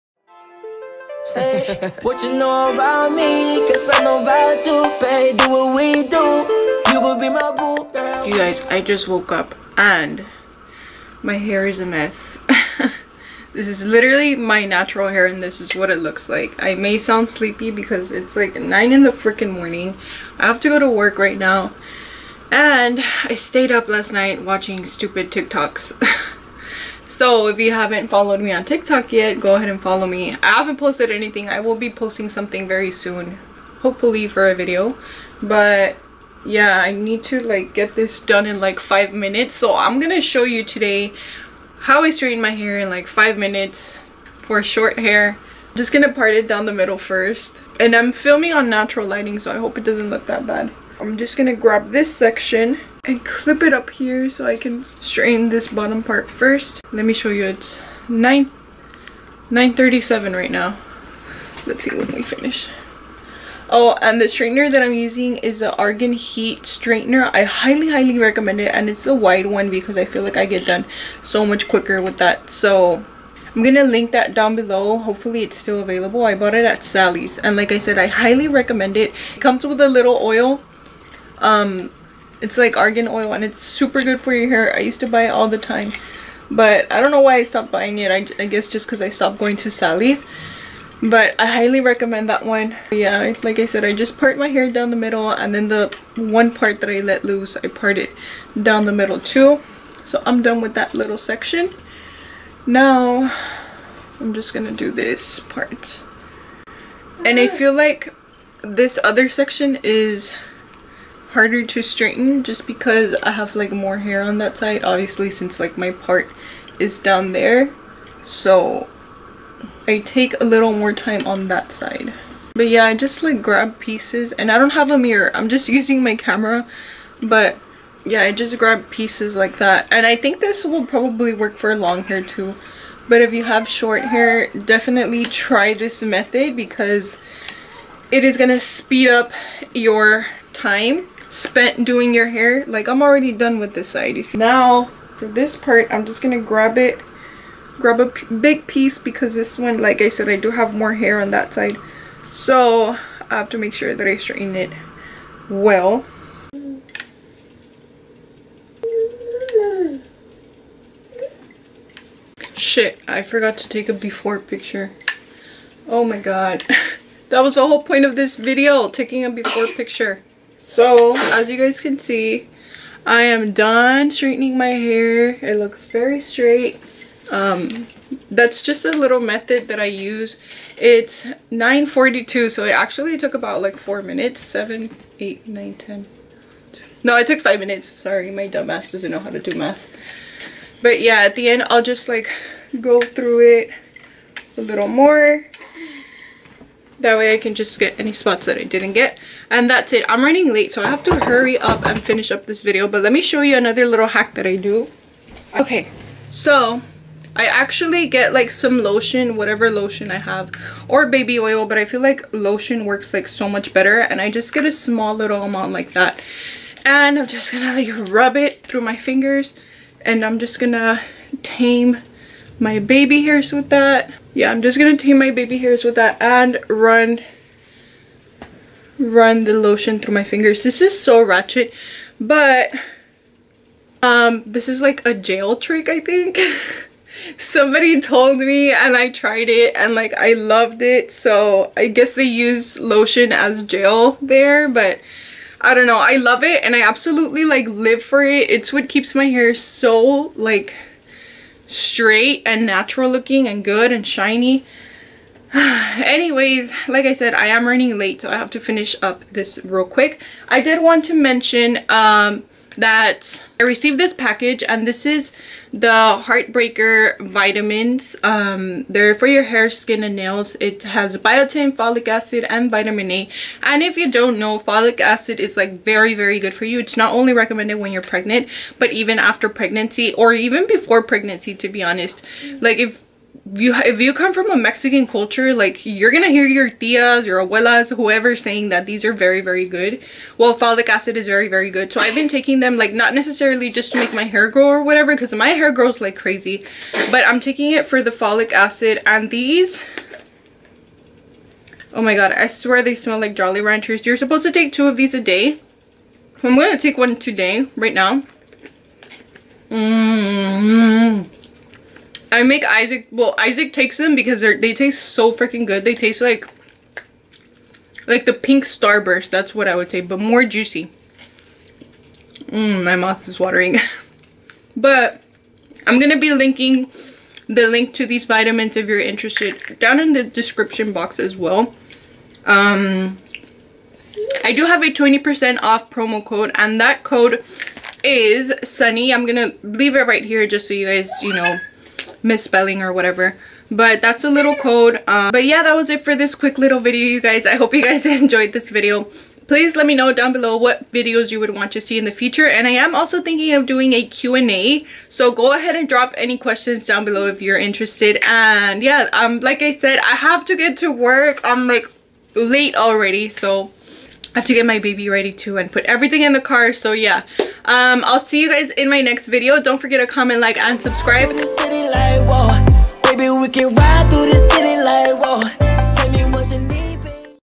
HOW TO STRAIGHTEN YOUR HAIR FAST! FOR SHORT HAIR!
Today I'm showing you guys how I straighten my short hair fast and easy. This is a quick tutorial on how I get my hair straight in 5 minutes, I swear this is the perfect routine to get a nice cute hairstyle for school time now. I love my short hair but I know some people may find it hard to straighten it or take longer but here's how I do mine fast and easy!

Here's the straightener I used:
(make sure it's the wide one to finish quicker lol)